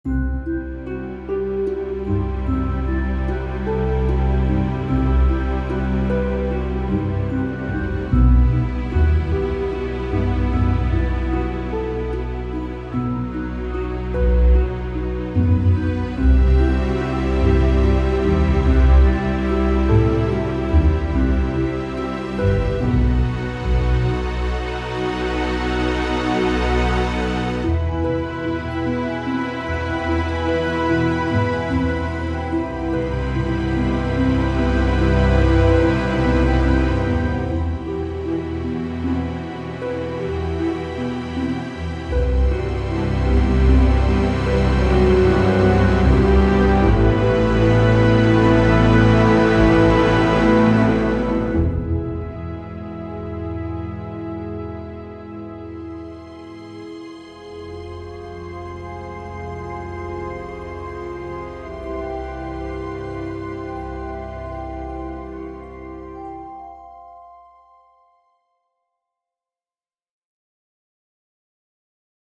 Albion ONE 10th Anniversary Edition Trailer (Rescore)
Spitfire Audio Albion One 10th Anniversary Edition Trailer Rescore (2018)
- Composition/Orchestration/Production by Jing Zhang